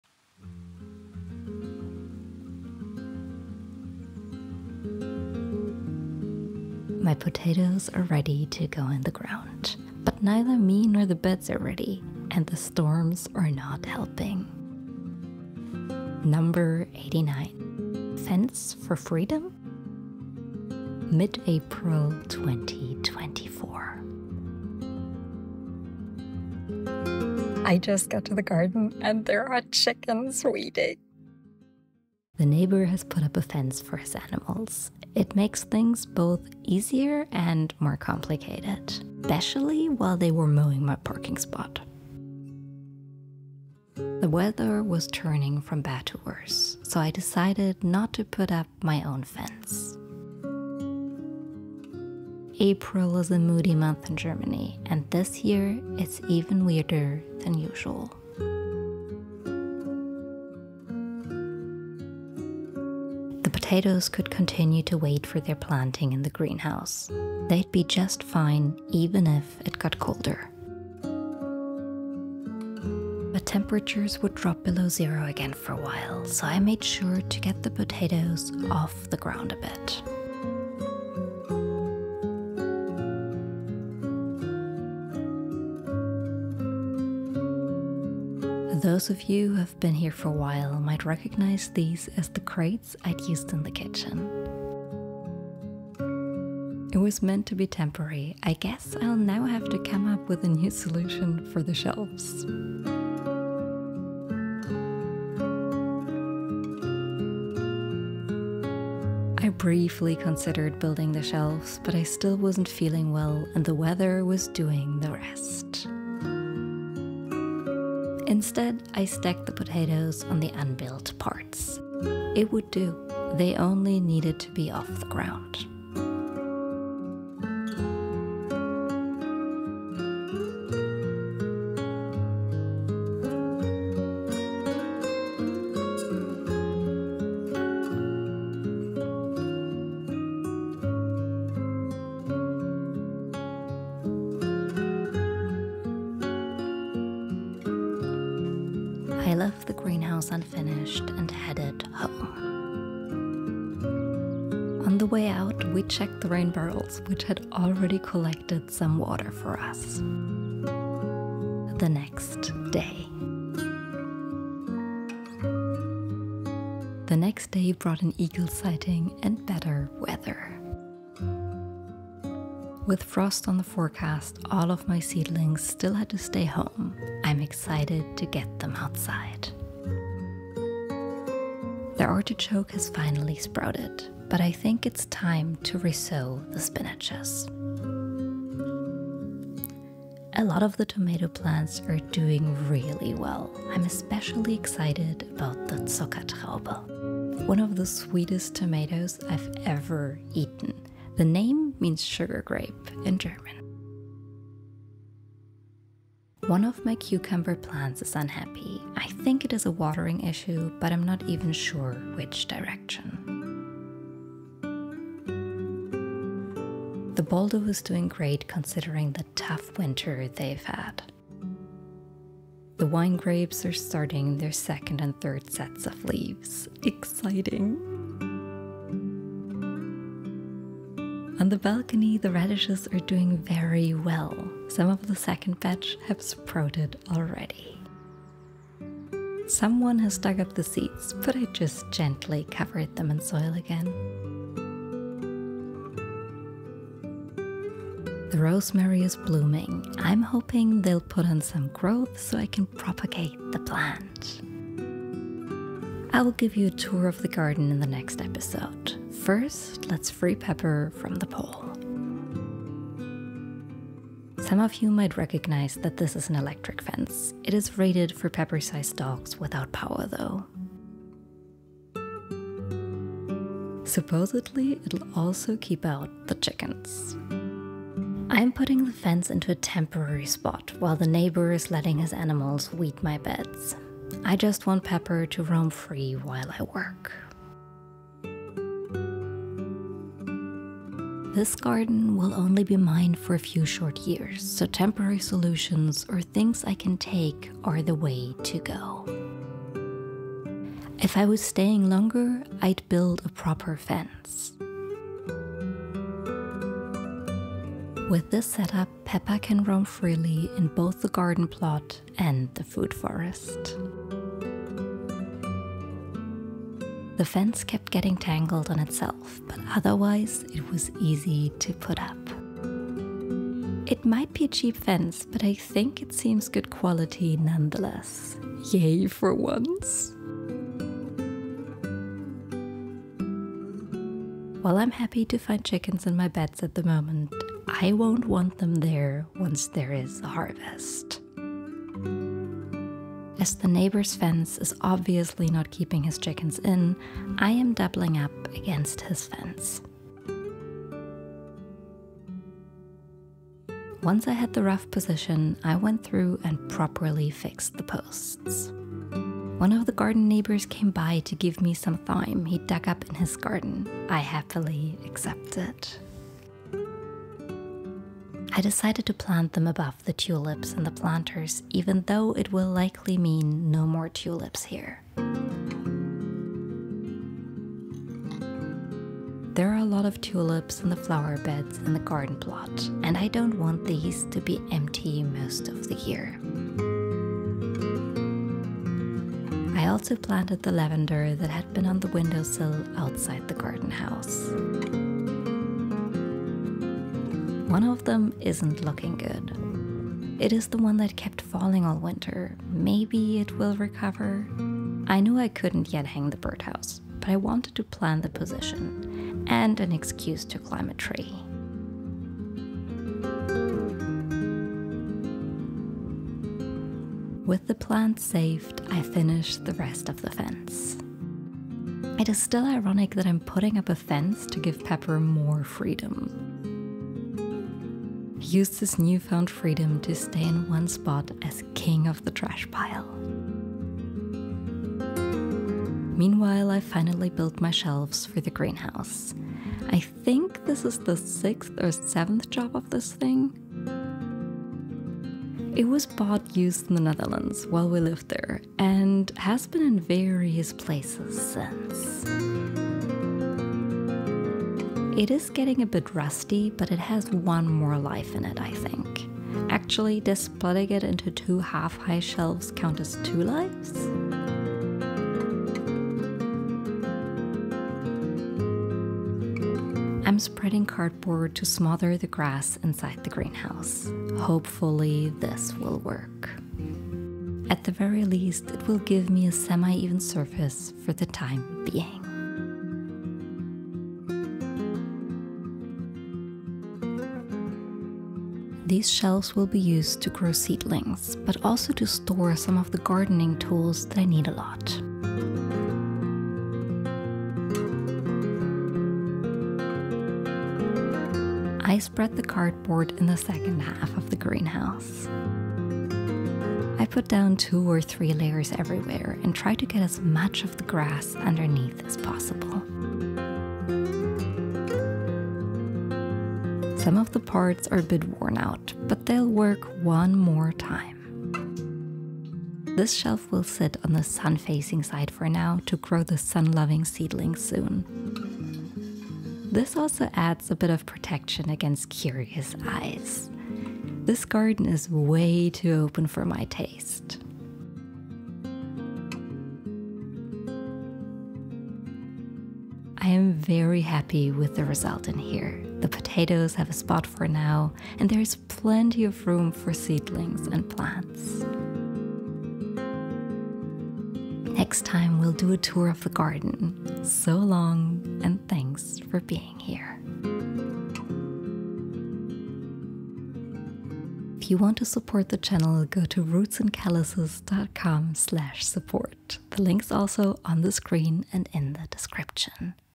A fence for the garden plot #089
My potatoes are ready to go in the ground. But neither me nor the bed are ready. And the storms are not helping. Ironically, I built a fence to give Pepper more freedom. The potatoes moved into the greenhouse, and I give you a tour of my first batch of seedlings.

FOOTAGE:
Drone footage was provided by my husband. All other footage was created by me or filmed for me.

CHAPTERS:
00:00 Introduction
00:31 The potatoes move
02:51 A seedling tour
04:15 A fence for Pepper
05:59 Planting thyme and lavender
07:13 Building greenhouse shelves